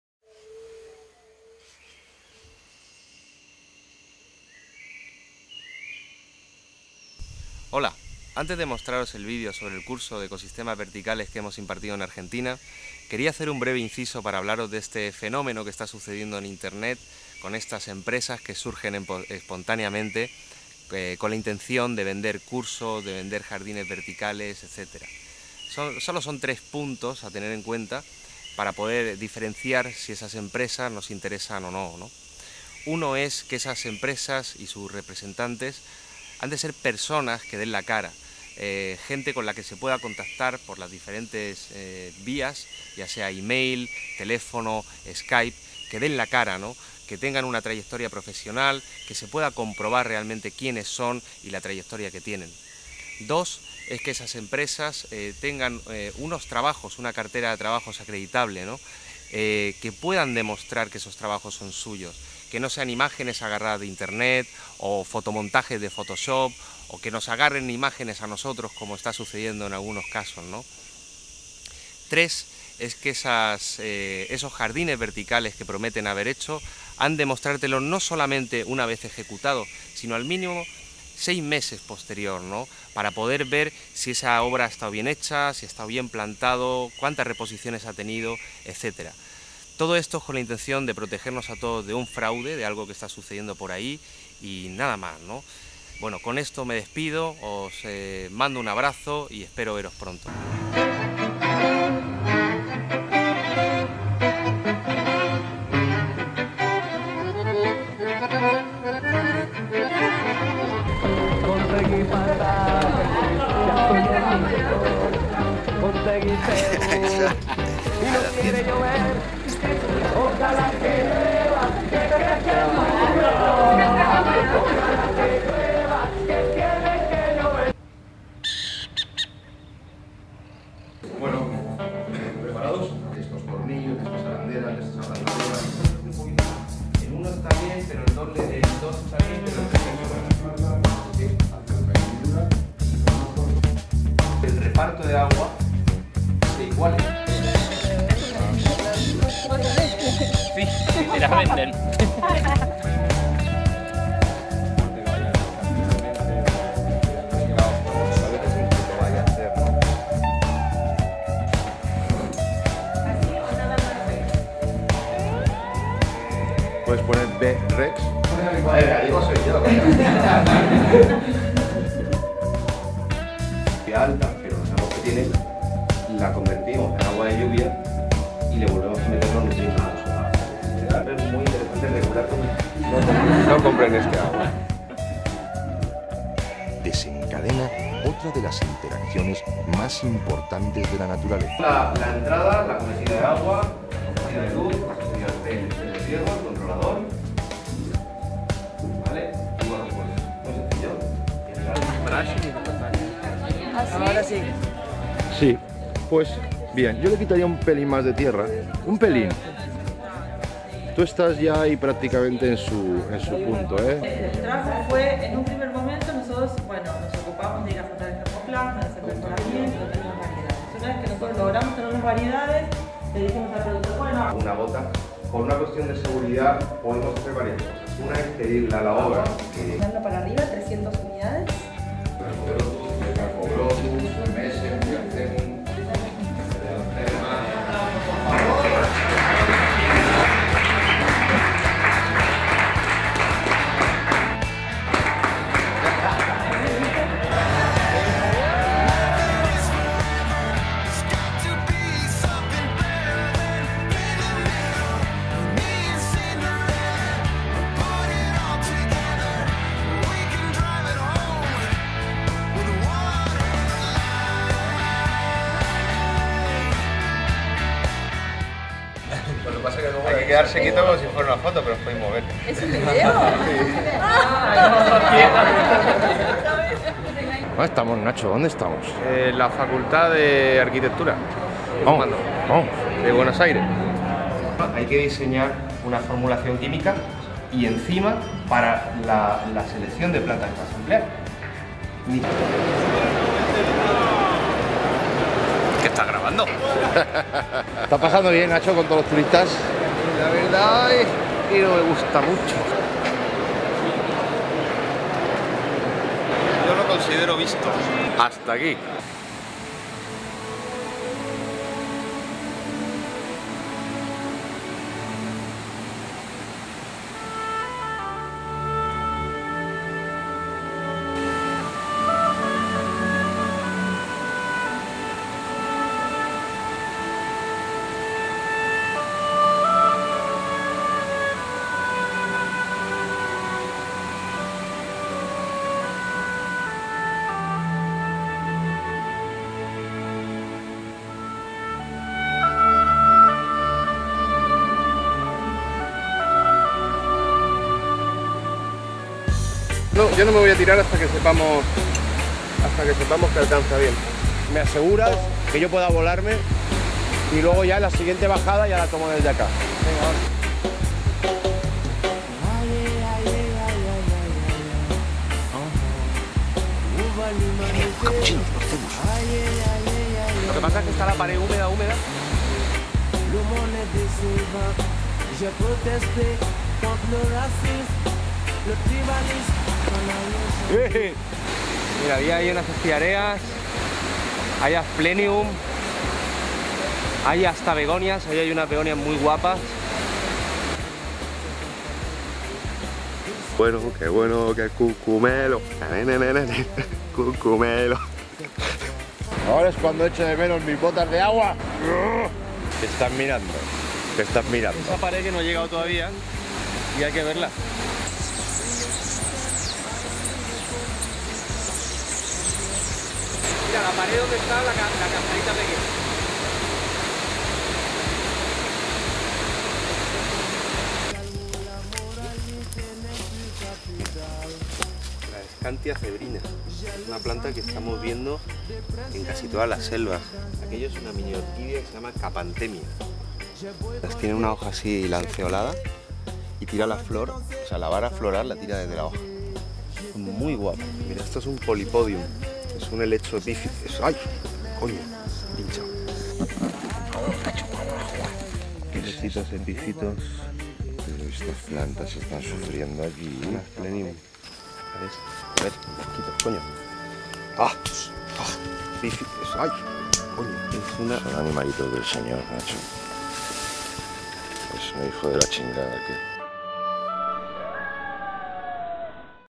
Vertical garden training course in Argentina. Advanced techniques. Paisajismo Urbano, 2012
Courses.

Second vertical gardening course teached by Nacho Solano, CEO of Paisajismo Urbano and Roberto Vidal in Argentina. 19 participants, 5 days, 25 hours of training, guest speakers, local plants studies, use of patents and licence.